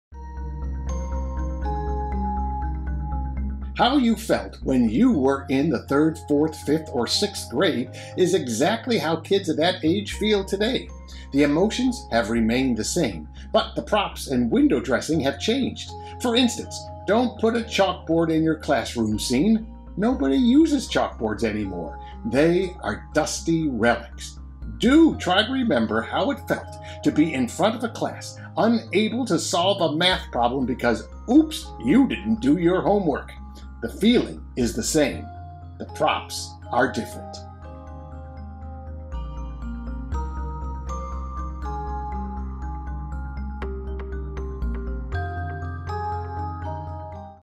How to Write a Mystery - Chris Grabenstein
Chris Grabenstein offers tips on unleashing your inner child